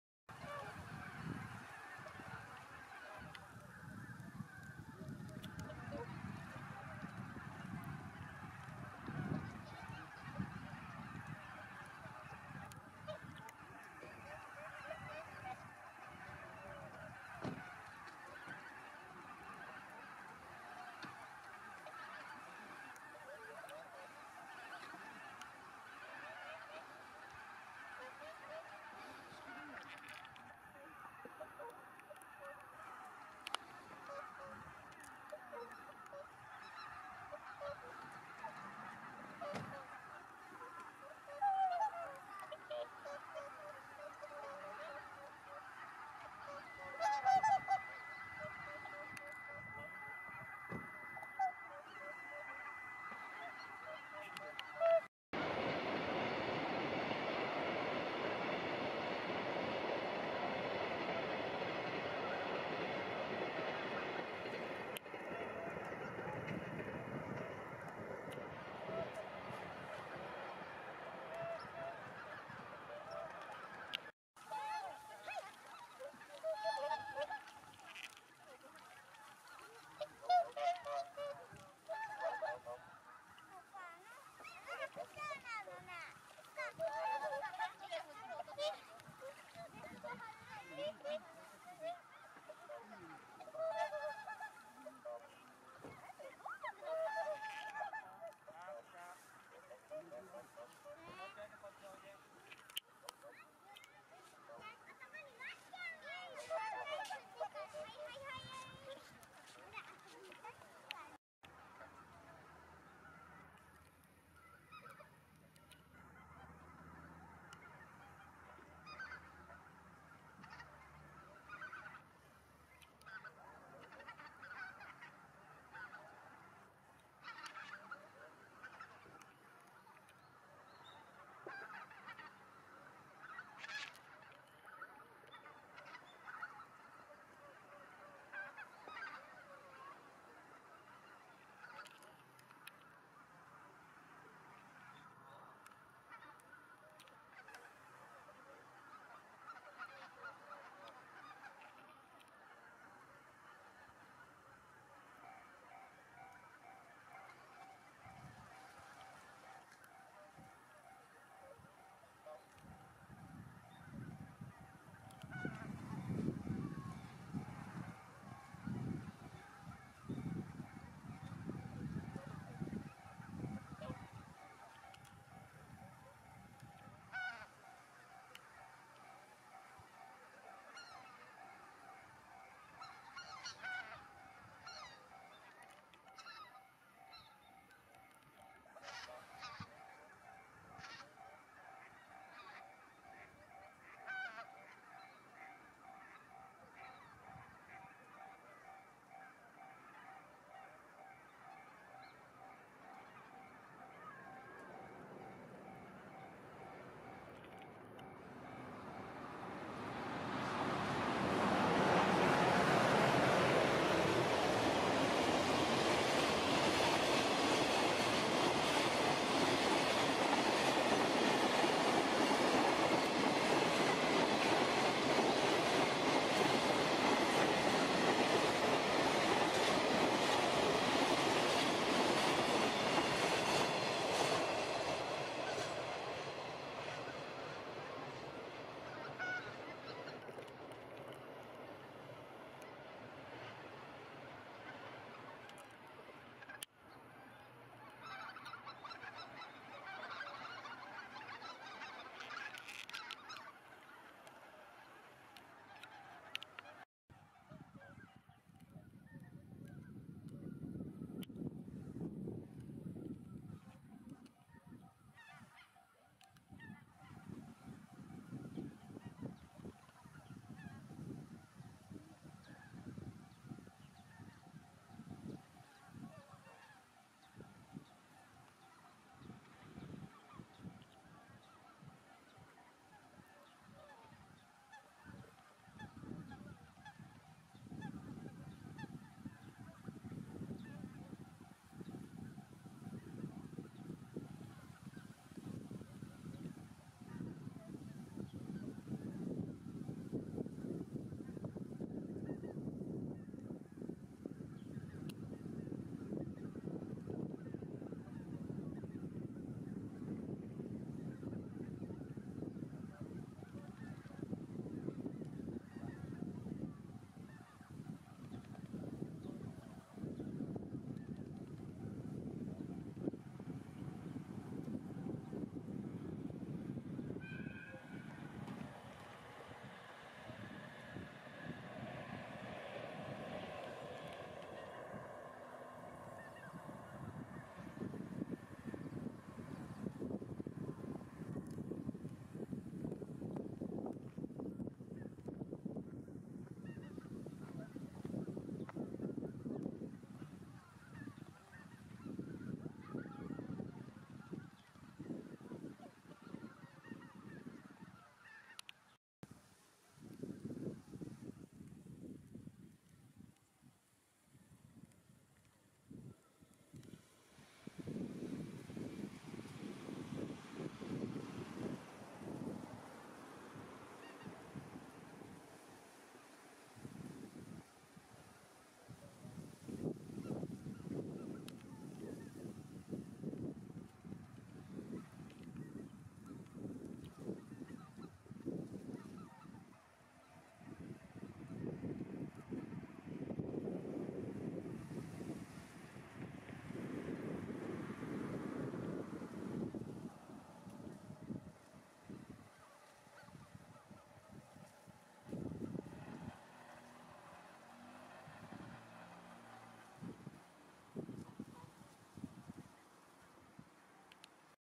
Swans, Geese and Trains in Japan
At the pond near Izunuma, Miyagi in Japan
伊豆沼の近くの池にて